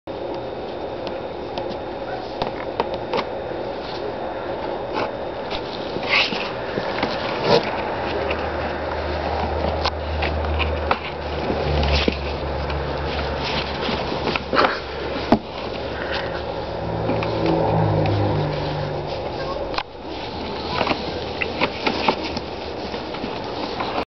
Raccoon Babies
We just got three baby raccoons today.